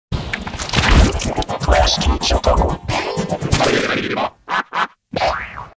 Soviet Cartoon Wolf Hates Klasky Csupo In G-Major 124 (Instructions In Description)
Note 2: It was made somewhere on 2017.

Note 3: This is not G-Major 124 the link below will show you.

How To Make G-Major 124:

(VideoFX)

Top Video:

1st: Compositing Mode: Difference

Solid Color: Cyan

Bottom Video:

1st: Pan/Crop - Flip Horizontal

2nd: Spherize - (Default) or Maximum Sphere In

(AudioFX)

elastique
Efficient

-5
-3
-1

Effect by ?